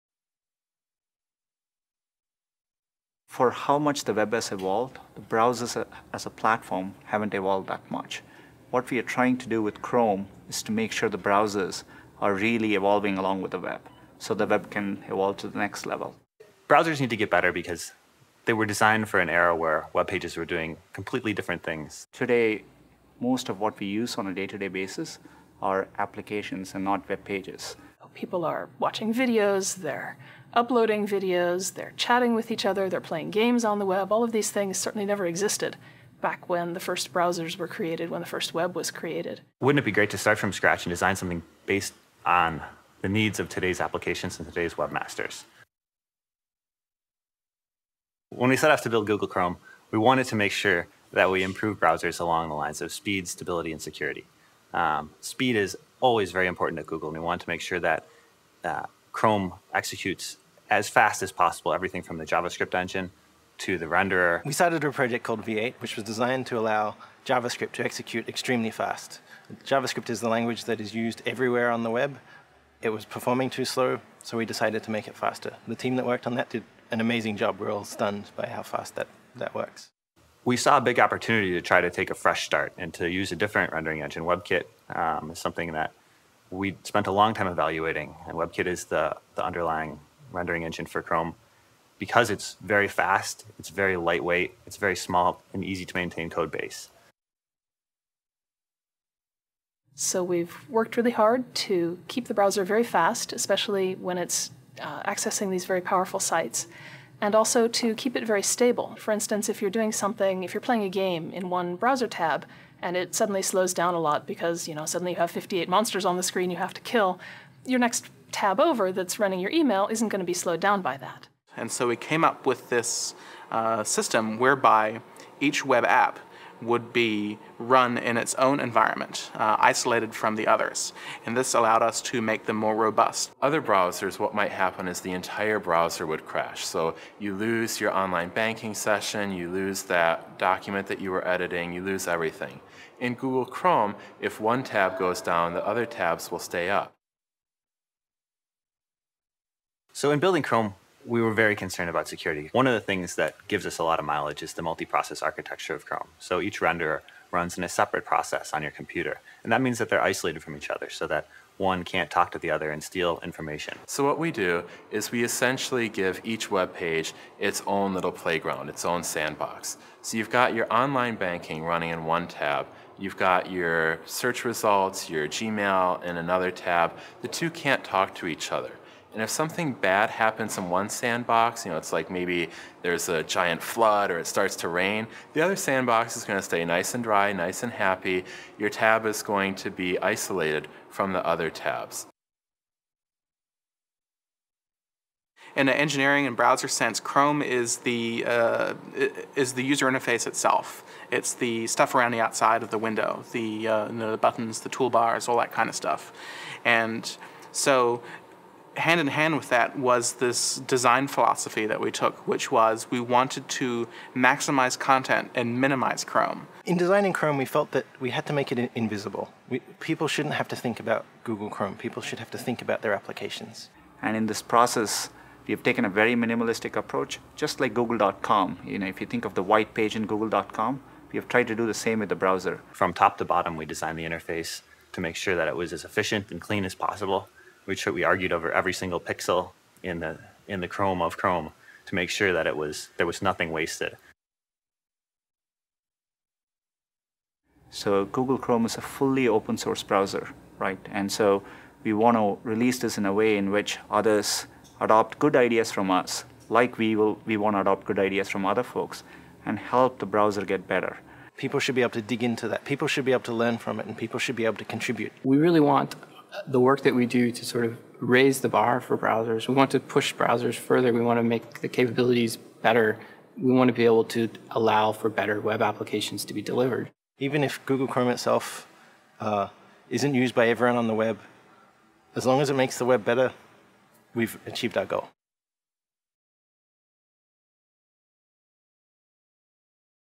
Die Story zu Google Chrome
Sehen Sie sich ein Video des Entwicklungsteams zu dem Konzept und den Funktionen an, die hinter Google Chrome stecken